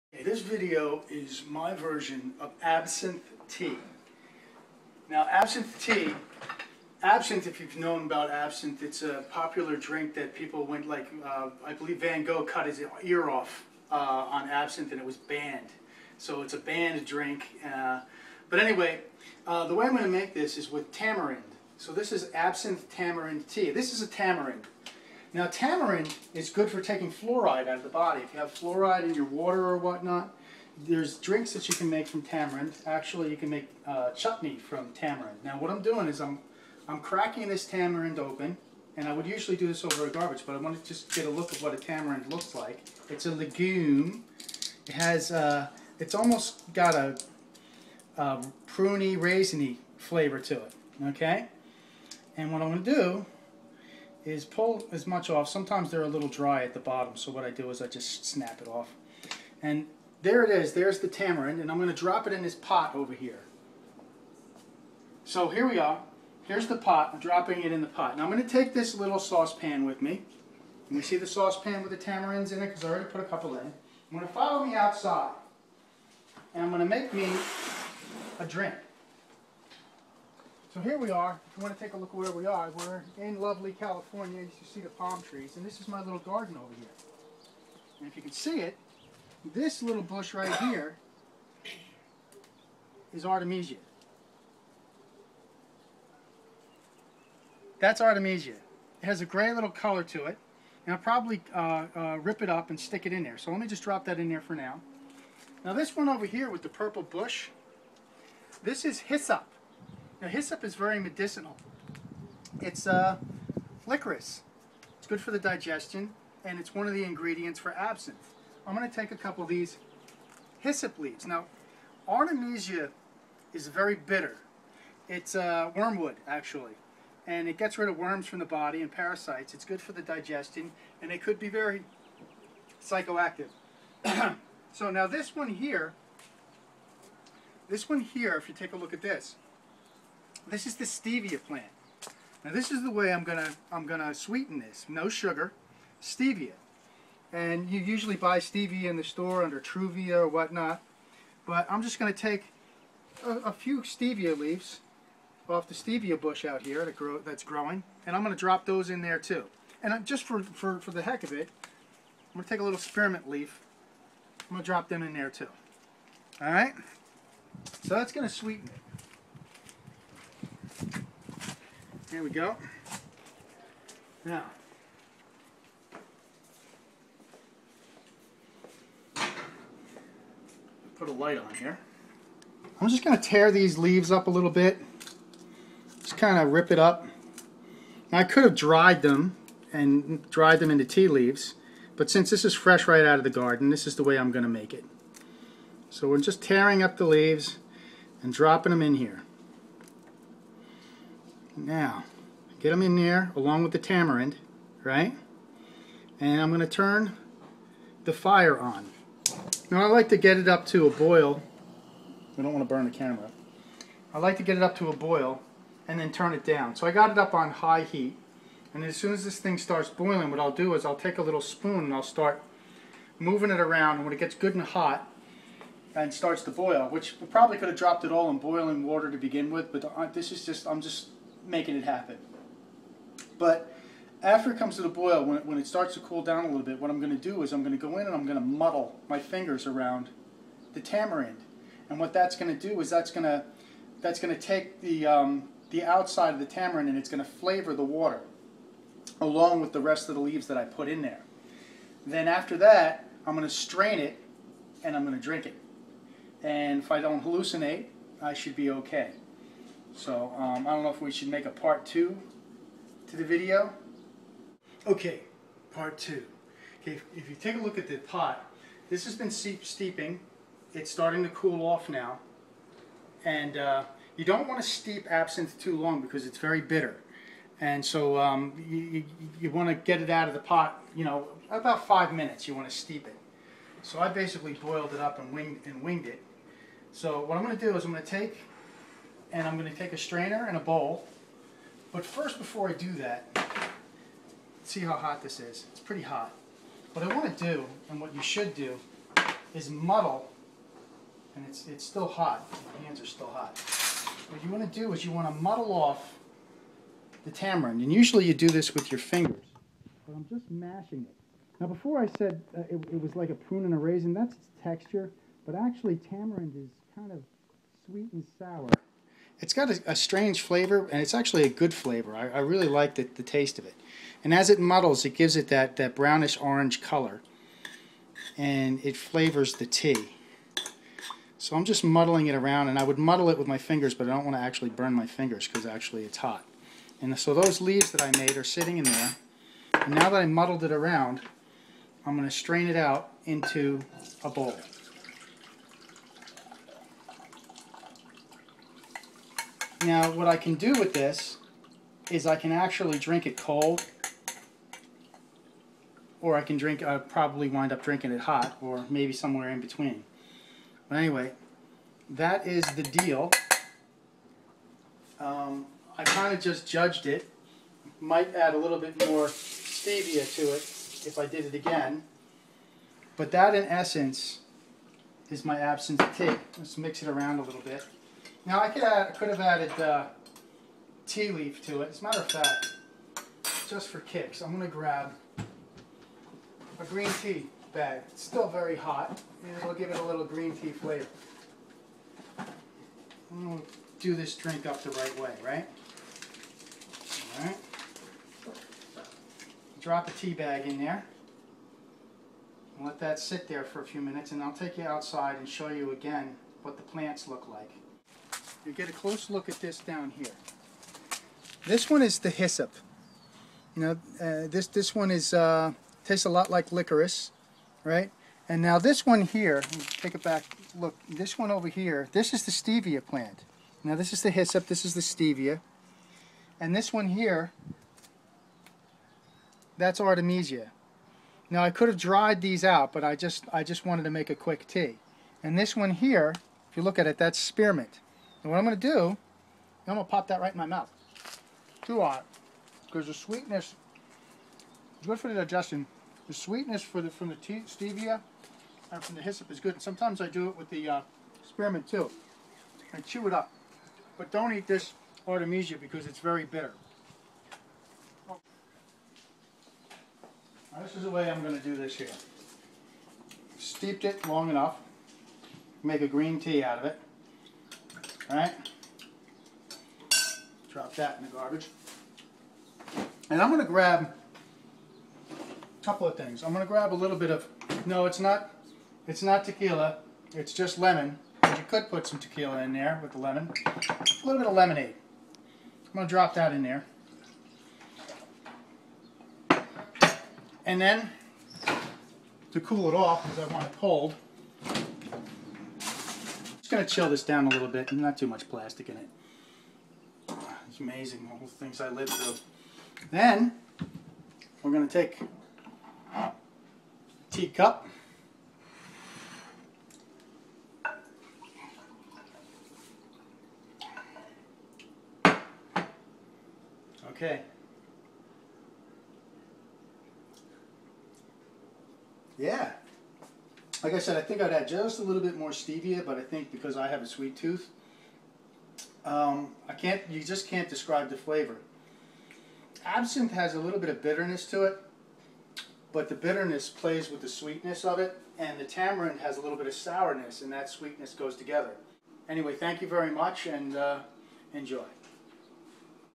TAMARIND ABSINTHE TEA
Making Tea with Tamarind & Artemesia Absinthia